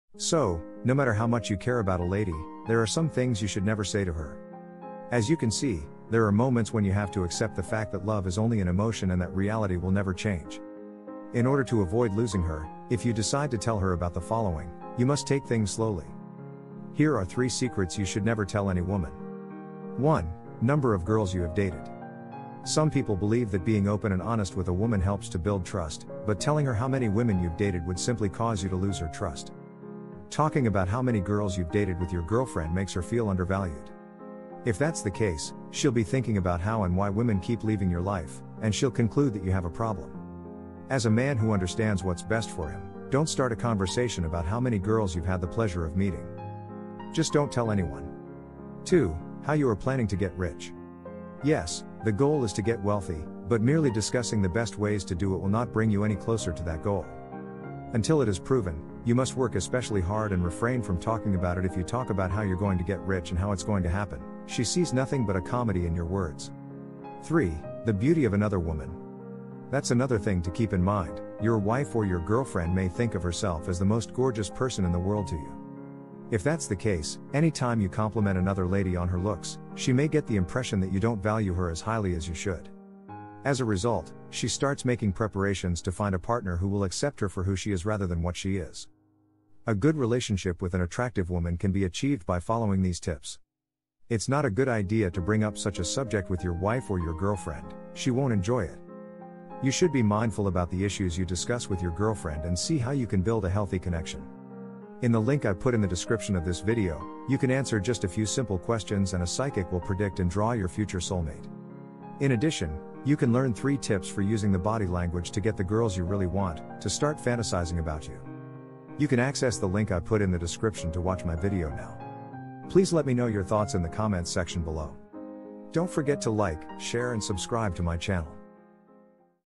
3 Secrets You Should Never Tell Any Woman
➤➤ Say These 3 Little Words To A Girl And She’ll Beg You To Be Her Fuck Buddy

US Tips For Men - 3 Secrets You Should Never Tell Any Woman
So, no matter how much you care about a lady, there are some things you should never say to her. As you can see, there are moments when you have to accept the fact that love is only an emotion and that reality will never change.
In order to avoid losing her, if you decide to tell her about the following, you must take things slowly.
Here are 3 Secrets you Should Never tell Any Woman

1: Number of girls you have dated.
Some people believe that being open and honest with a woman helps to build trust, but telling her how many women you’ve dated would simply cause you to lose her trust.
Talking about how many girls you’ve dated with your girlfriend makes her feel undervalued.
If that’s the case, she’ll be thinking about how and why women keep leaving your life, and she’ll conclude that you have a problem.
As a man who understands what’s best for him, don’t start a conversation about how many girls you’ve had the pleasure of meeting. Just don’t tell anyone.

2: How you are planning to get rich.
Yes, the goal is to get wealthy, but merely discussing the best ways to do it will not bring you any closer to that goal. Until it is proven, you must work especially hard and refrain from talking about it If you talk about how you’re going to get rich and how it’s going to happen, she sees nothing but a comedy in your words.

3: The beauty of another woman.
That’s another thing to keep in mind: Your wife or your girlfriend may think of herself as the most gorgeous person in the world to you.
If that’s the case, any time you compliment another lady on her looks, she may get the impression that you don’t value her as highly as you should.
As a result, she starts making preparations to find a partner who will accept her for who she is rather than what she is.
A good relationship with an attractive woman can be achieved by following these tips. It’s not a good idea to bring up such a subject with your wife or your girlfriend; she won’t enjoy it.
You should be mindful about the issues you discuss with your girlfriend and see how you can build a healthy connection.

Hagtag